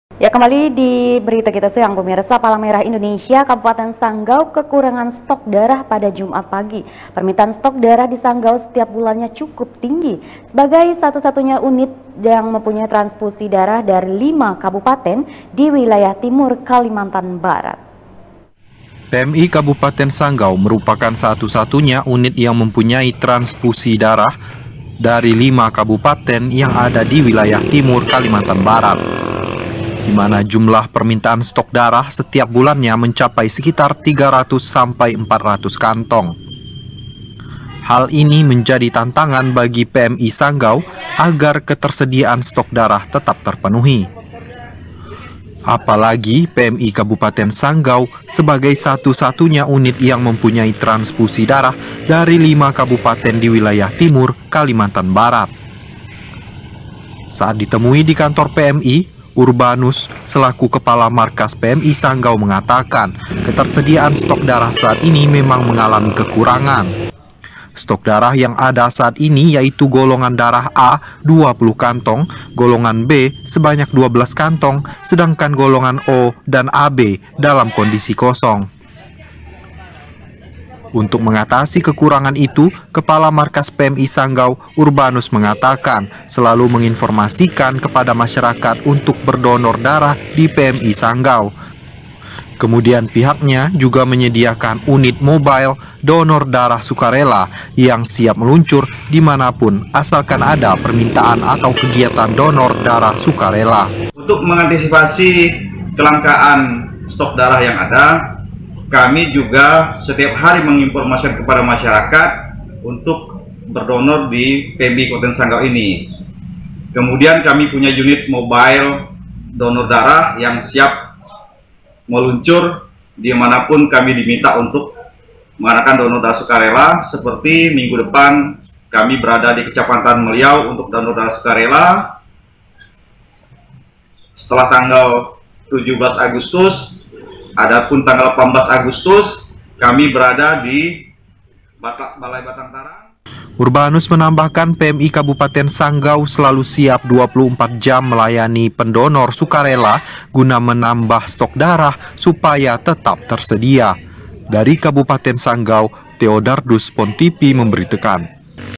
PMI Kabupaten Sanggau Kekurangan Stok Darah
Palang Merah Indonesia Kabupaten Sanggau kekurangan stok darah, pada Jumat pagi. Permintaan stok darah di Sanggau setiap bulannya cukup tinggi, sebagai satu satunya unit yang mempunyai tranfusi darah dari lima kabupaten di wilayah timur, Kalimantan Barat